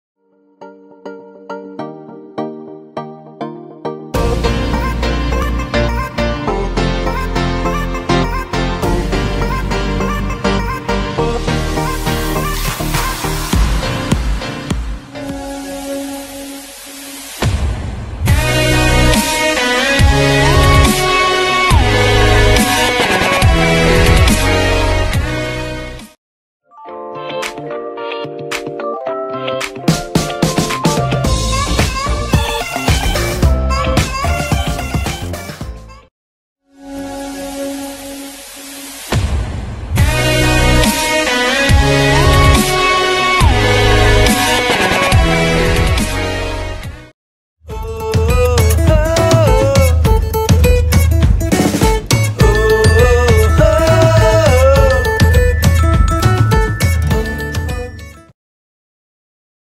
Dji Mini 2 Quick Shot Test
Tested out the Quick Shot modes on my Dji Mini2. Can’t wait to travel now and capture some great moments.